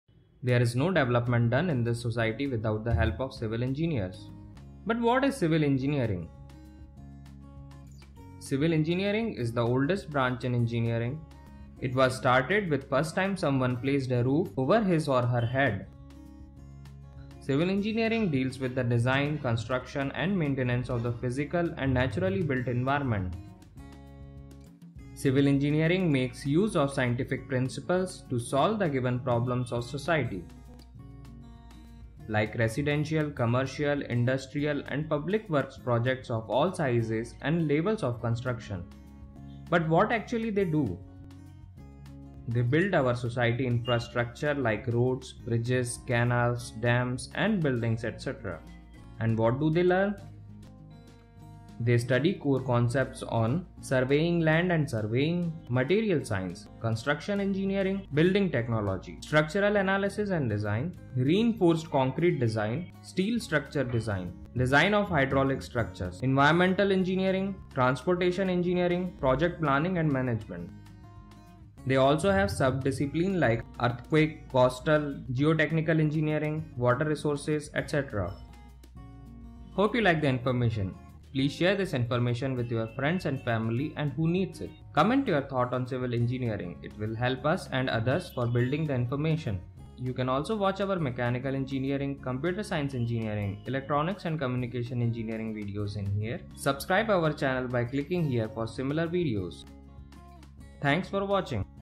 Introduction To CIVIL Engineering
In this video we are going to discuss about how What is Civil Engineering. What Civil Engineers do and what actually they study. This will give you brief idea of Civil Engineers and many sub discipline they work.